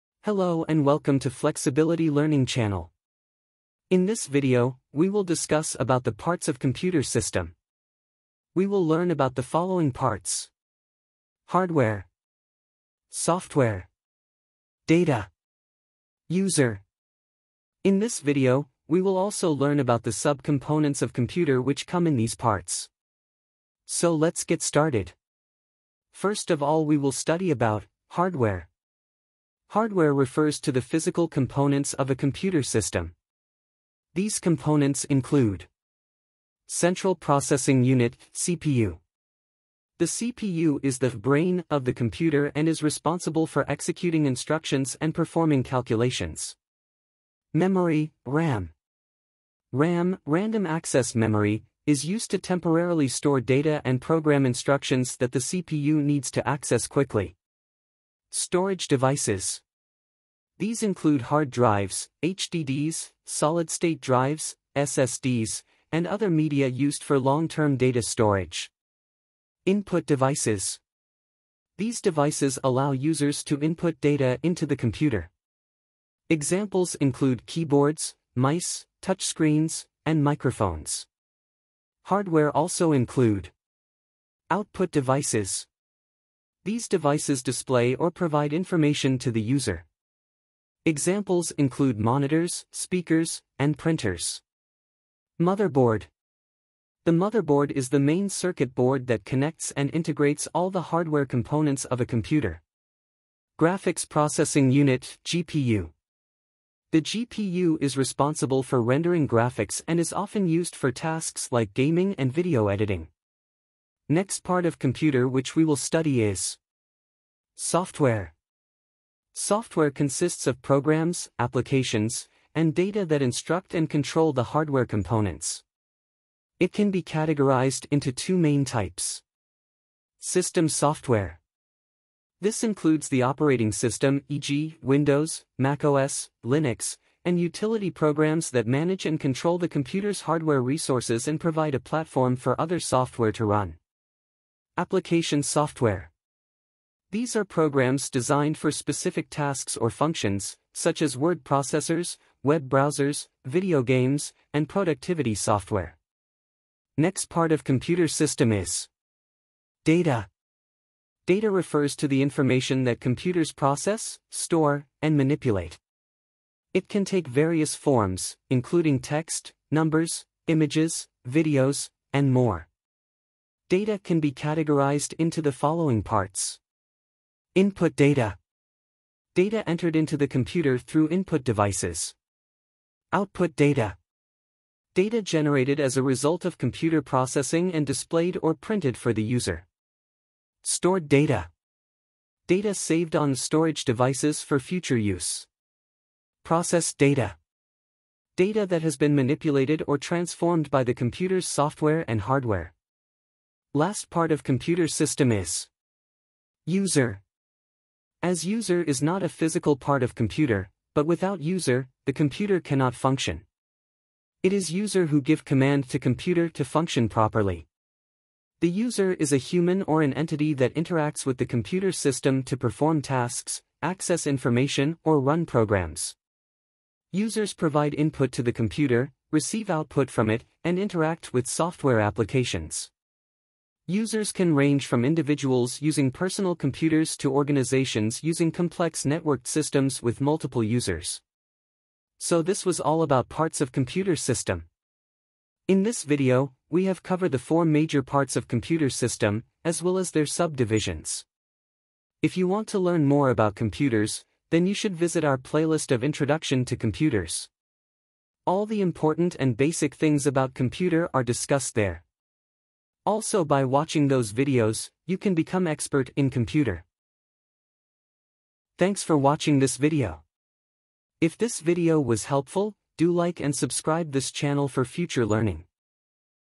Parts Of Computer | Components Of Computer | All Parts Of Computer(Hardware, Software, Data, User)
Welcome to our informative video where we dive deep into the heart of computer systems. In this video, we'll break down the four fundamental components of computers and explore their intricate subparts to help you gain a better understanding of how these incredible machines work.

Hardware: We'll start by delving into the physical elements of a computer. From the central processing unit (CPU) to memory, storage devices, and input/output peripherals, we'll explain the roles of each hardware component and how they interact to process information.

Hardware refers to the physical components of a computer system. These components include:
Central Processing Unit (CPU): The CPU is the "brain" of the computer and is responsible for executing instructions and performing calculations.

Memory (RAM): RAM (Random Access Memory) is used to temporarily store data and program instructions that the CPU needs to access quickly.

Storage Devices: These include hard drives (HDDs), solid-state drives (SSDs), and other media used for long-term data storage.

Input Devices: These devices allow users to input data into the computer. Examples include keyboards, mice, touchscreens, and microphones.

Output Devices: These devices display or provide information to the user. Examples include monitors, speakers, and printers.

Motherboard: The motherboard is the main circuit board that connects and integrates all the hardware components of a computer.

Graphics Processing Unit (GPU): The GPU is responsible for rendering graphics and is often used for tasks like gaming and video editing.

Software: Next, we'll demystify the world of software. Learn about the distinction between system software and application software, and how these pieces work together to enable the computer to perform various tasks. We'll also touch on the importance of operating systems and how they act as the bridge between hardware and software.

Software consists of programs, applications, and data that instruct and control the hardware components. It can be categorized into two main types:
System Software: This includes the operating system (e.g., Windows, macOS, Linux) and utility programs that manage and control the computer's hardware resources and provide a platform for other software to run.
Application Software: These are programs designed for specific tasks or functions, such as word processors, web browsers, video games, and productivity software.

Data: Data is the lifeblood of any computer system, and in this segment, we'll discuss how computers store, manage, and manipulate data. From bits and bytes to file systems and databases, we'll clarify the journey of data within a computer.

Data refers to the information that computers process, store, and manipulate. It can take various forms, including text, numbers, images, videos, and more. Data can be categorized as:
Input Data: Data entered into the computer through input devices.
Output Data: Data generated as a result of computer processing and displayed or printed for the user.
Stored Data: Data saved on storage devices for future use.
Processed Data: Data that has been manipulated or transformed by the computer's software and hardware.

User: The user is the driving force behind computer systems, and we'll explain how humans interact with computers through user interfaces. From graphical user interfaces (GUIs) to command-line interfaces (CLIs), we'll discuss the ways users communicate with their machines.

The user is a human or an entity that interacts with the computer system to perform tasks, access information, or run programs.
Users provide input to the computer, receive output from it, and interact with software applications.
Users can range from individuals using personal computers to organizations using complex networked systems with multiple users.

Throughout the video, we'll use clear and simple explanations, engaging visuals, and real-world examples to ensure that you leave with a solid grasp of these essential computer components. Whether you're a tech enthusiast looking to expand your knowledge or a beginner seeking to demystify the world of computers, this video is your one-stop guide to understanding the core components of these incredible machines.